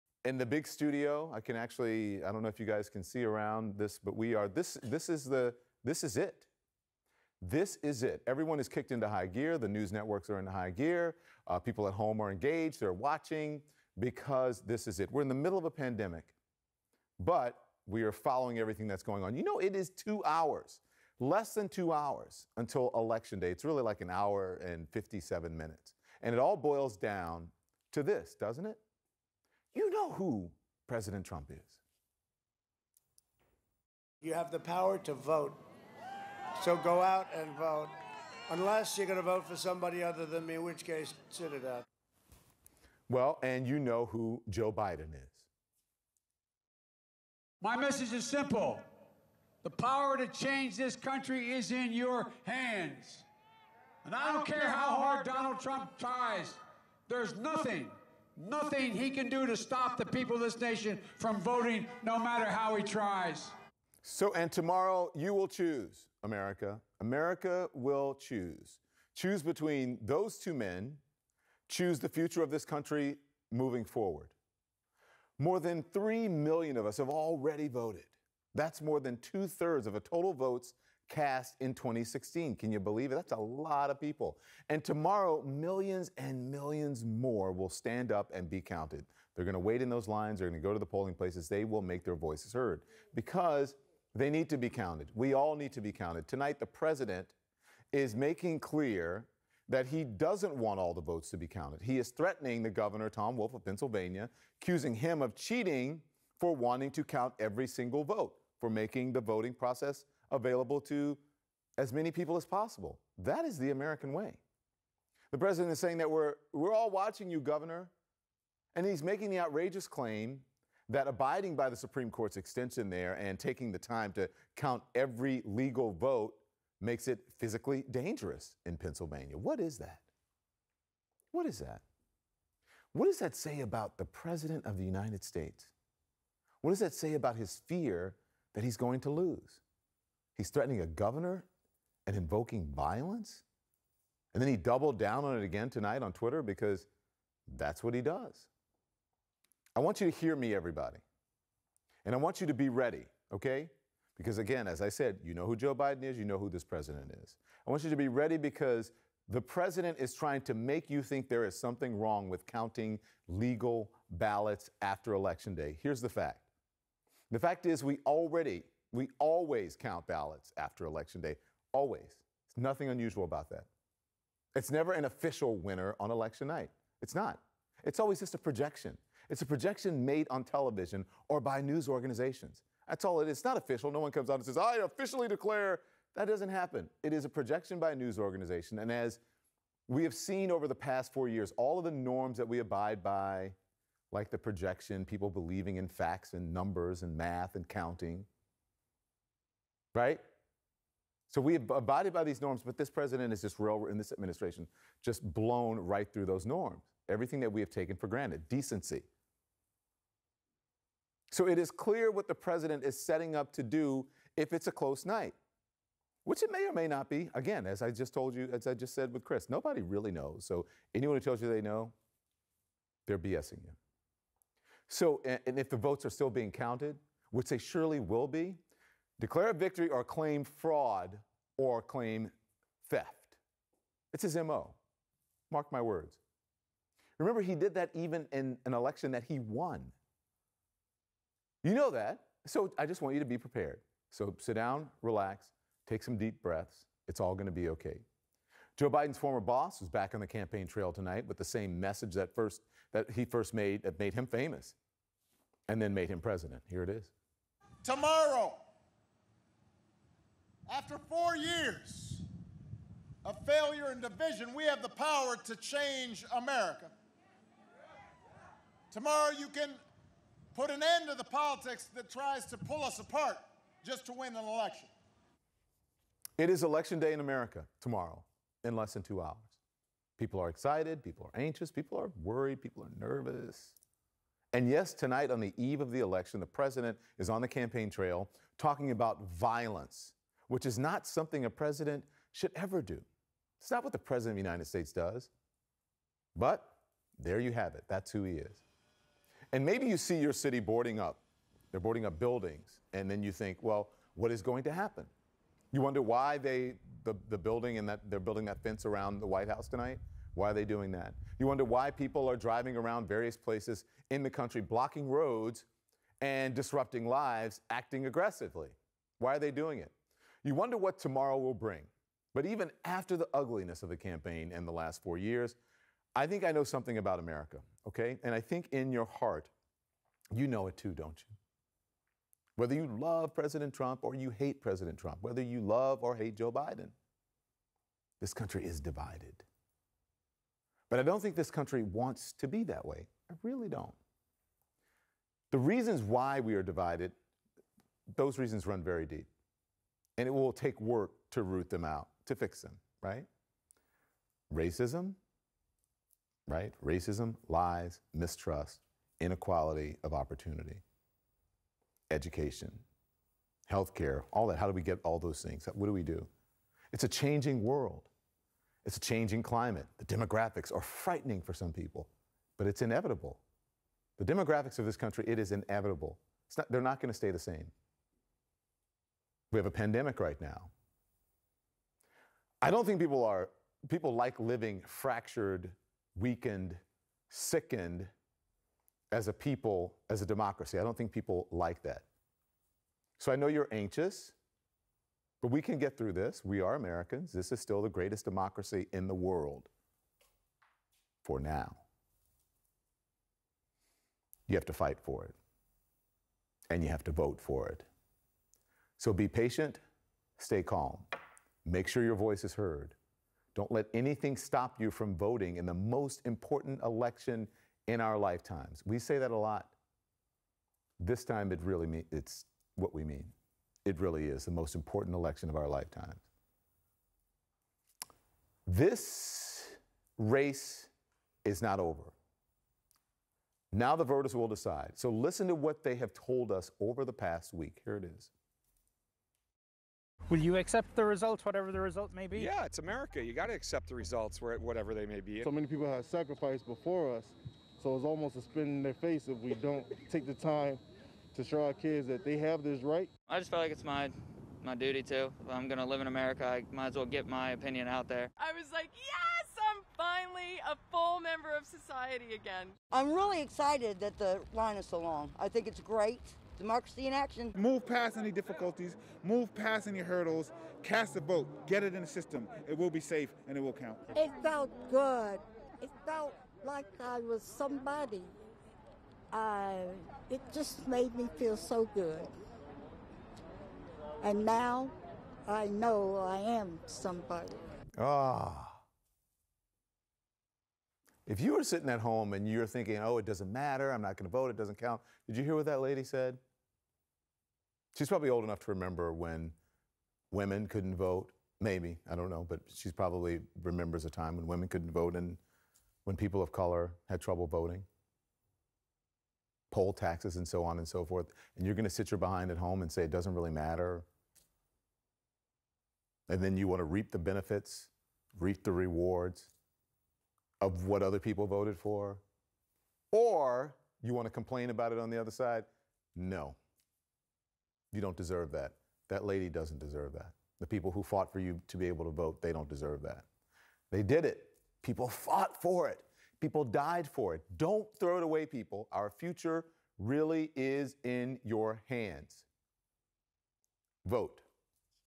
Don Lemon: The reasons why US is divided run very deep
CNN's Don Lemon discusses the importance of the 2020 presidential election between President Trump and Democratic nominee Joe Biden when the country is in a pandemic and deeply divided.